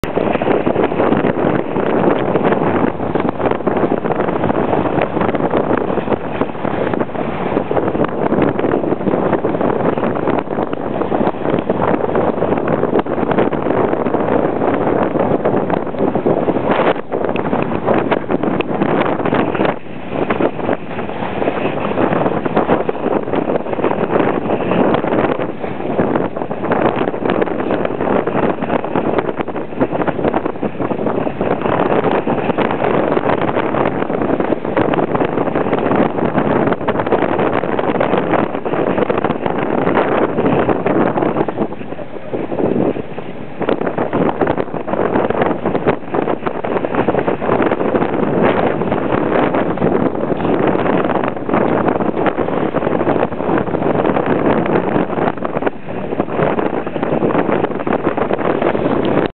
sailing in rough wind
sailing with Etap 28 and 30 knots of wind